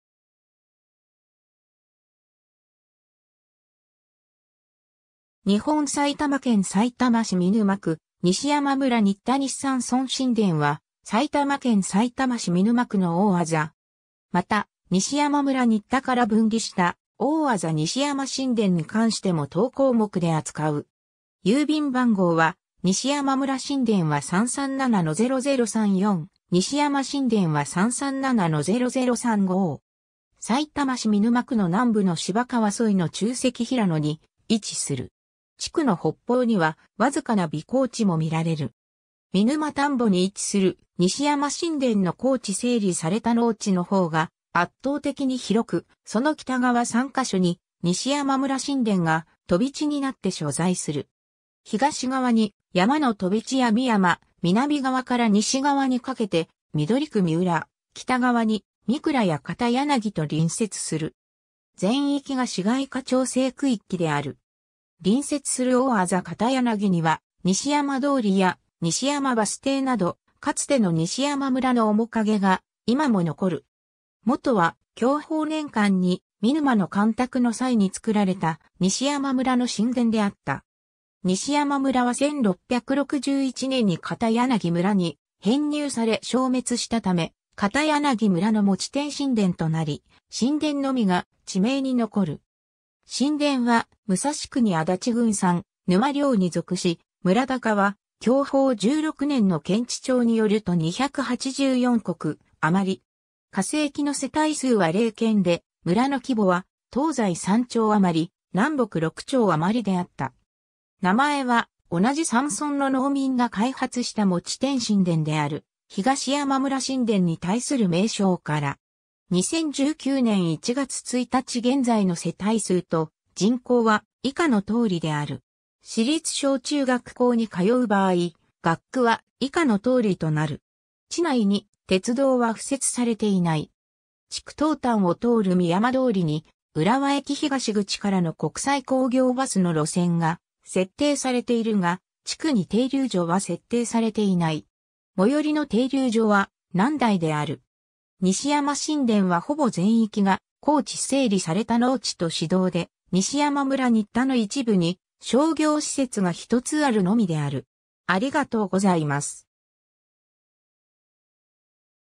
西山村新田
見沼区の町・字
日本 」 埼玉県 」 さいたま市 」 見沼区 」 西山村新田 西山村新田（にしやまむらしんでん）は、埼玉県さいたま市見沼区の大字。
また、西山村新田から分離した、大字西山新田（にしやましんでん）に関しても当項目で扱う。
郵便番号は西山村新田は337-0034、西山新田は337-0035。
 さいたま市見沼区の南部の芝川沿いの沖積平野に位置する。
地区の北方には僅かな微高地も見られる。
見沼田圃に位置する西山新田の耕地整理された農地の方が圧倒的に広く、その北側3ヶ所に西山村新田が飛地になって所在する。
東側に山の飛地や見山、南側から西側にかけて緑区三浦、北側に御蔵や片柳と隣接する。
全域が市街化調整区域である。
 隣接する大字片柳には、「西山通り」や「西山」バス停など、かつての西山村の面影が今も残る。
 もとは享保年間に見沼の干拓の際につくられた西山村（山村を二分する西側の西山分）の新田であった。
西山村は1661年（寛文元年）に片柳村（現在の大字片柳西部）に編入され消滅した為、片柳村の持添新田となり新田のみが地名に残る。
新田は武蔵国足立郡三沼領に属し、村高は享保16年の検地帳によると284石余。
化政期の世帯数は0軒で、村の規模は東西3町余、南北6町余であった。
名前は同じ山村（東側の東山分である東山村）の農民が開発した持添新田である東山村新田に対する名称から。
 2019年（平成31年）1月1日現在の世帯数と人口は以下の通りである。
 地内に鉄道は敷設されていない。
 地区東端を通る見山通りに浦和駅東口からの国際興業バスの路線（浦08および浦08-2）が設定されているが、地区に停留所は設定されていない。
最寄りの停留所は「南台」である。
 西山新田はほぼ全域が耕地整理された農地と市道で、西山村新田の一部に商業施設が一つあるのみである。